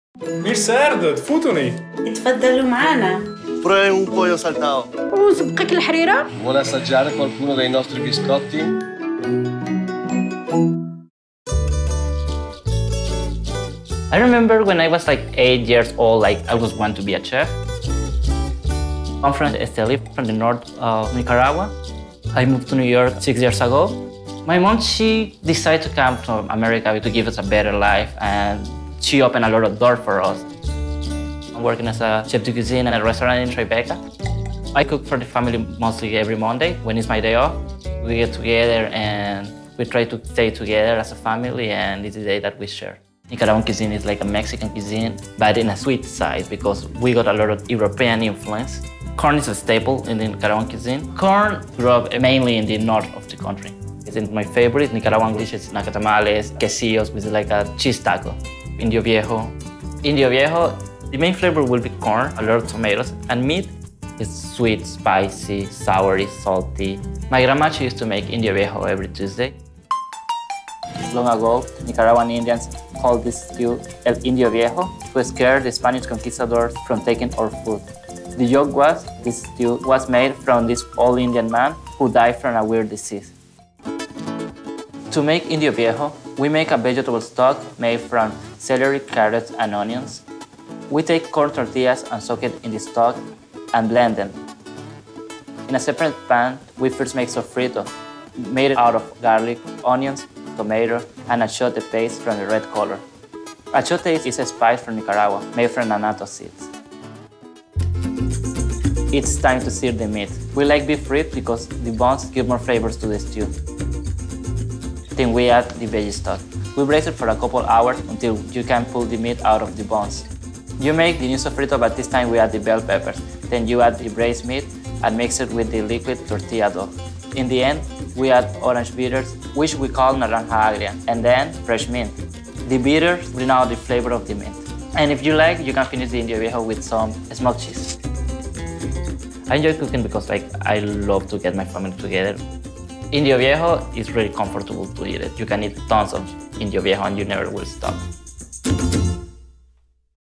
Native Dish: Nicaraguan Indio Viejo Stew - NYC Immigrant Cuisine: Javier Álvarez
Ever since the tender age of 8, Estelí, Nicaraguan-native Javier Álvarez knew that cooking would be his destiny.  Six years later, after coming to New York City, his dream would become reality as Chef de Cuisine at Tribeca's Graffiti Earth Restaurant, led by restaurateur Jehangir Metha (of TV Iron Chef fame).

His mother, Mercedes Arroglia, came to the United States in 2004 from a family of Tobacco workers--as Nicaragua is one of the world's renowned producers of Cigars--to continue her expertise in working with Autistic children and to provide better opportunity for her children.  Javier, fresh out of Nicaraguan culinary school, seized the opportunity and came to New York to build his career.

On his days off, Javier gathers his family at their Brighton Beach home and cooks for them his grandmother's recipe of El Indio Viejo--a savory cornmeal pozole stew containing tomatoes, beef ribs, mint and a hint of orange bitters.  It's a legendary recipe that even ancient Conquistadors who ruled old Nicaragua would dare not eat!

Ivan Peña, Dir. of Photography

Wikipedia Authors Nsaum75, Dr d12